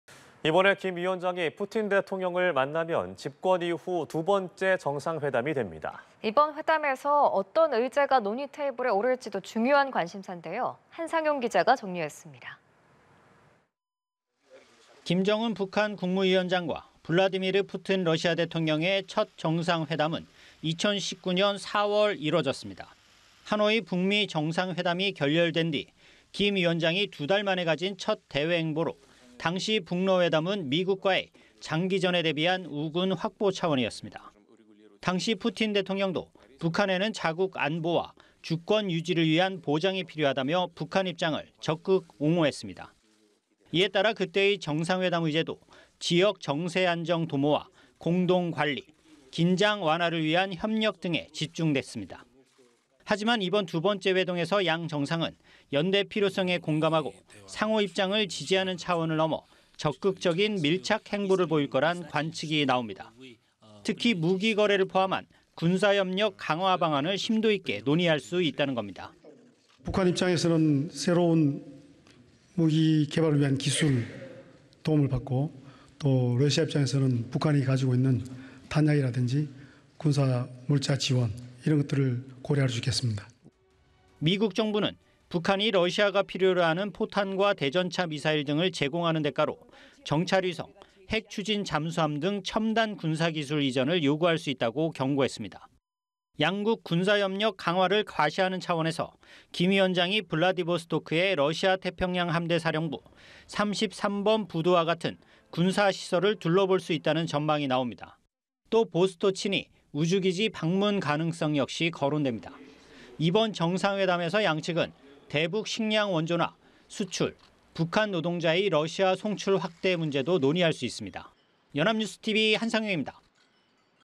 북·러 정상회담 의제는…김정은, 군사시설 방문 가능성 / 연합뉴스TV (YonhapnewsTV)
북·러 정상회담 의제는…김정은, 군사시설 방문 가능성
[뉴스리뷰]

[앵커]

김정은 북한 국무위원장과 푸틴 러시아 대통령의 정상회담이 열릴 것으로 공식 확인됐습니다.

이번에 김 위원장이 푸틴 대통령을 만나면 집권 이후 두 번째 정상회담이 됩니다.

이번 회담에서 어떤 의제가 논의 테이블에 오를지도 중요한 관심사인데요.

한상용 기자가 정리했습니다.

[기자]

김정은 북한 국무위원장과 블라디미르 푸틴 러시아 대통령의 첫 정상회담은 2019년 4월에 이뤄졌습니다.

하노이 북미정상회담이 결렬된 뒤 김 위원장이 두 달 만에 가진 첫 대외 행보로, 당시 북러 회담은 미국과의 장기전에 대비한 우군 확보 차원이었습니다.

푸틴 대통령도 "북한에는 자국 안보와 주권 유지를 위한 보장이 필요하다"며 북한 입장을 적극 옹호했습니다.

이에 따라 그 때의 정상회담 의제도 '지역 정세안정 도모와 공동 관리', '긴장 완화를 위한 협력' 등에 집중됐습니다.

하지만, 이번 두 번째 회동에서 양 정상은 연대 필요성에 공감하고 상호 입장을 지지하는 차원을 넘어 적극적인 밀착 행보를 보일 거란 관측이 나옵니다.

특히 무기 거래를 포함한 군사협력 강화 방안을 심도 있게 논의할 수 있다는 겁니다.

[이종섭 / 국방부 장관] "북한 입장에서는 새로운 무기 개발을 위한 기술 도움을 받고, 러시아 입장에서는 북한이 가지고 있는 탄약이라든지 군사 물자 지원, 이런 것들을 고려할 수 있겠습니다."

미국 정부는 북한이 러시아가 필요로 하는 포탄과 대전차 미사일 등을 제공하는 대가로 정찰위성, 핵추진잠수함 등 첨단 군사기술 이전을 요구할 수 있다고 경고했습니다.

양국 군사협력 강화를 과시하는 차원에서 김 위원장이 블라디보스토크의 러시아태평양함대사령부 33번 부두와 같은 군사시설을 둘러볼 수 있다는 전망이 나옵니다.

또 보스토치니 우주기지 방문 가능성 역시 거론됩니다.

이번 정상회담에서 양측은 대북 식량 원조나 수출 확대, 북한 노동자의 러시아 송출 확대 문제도 논의할 수 있습니다.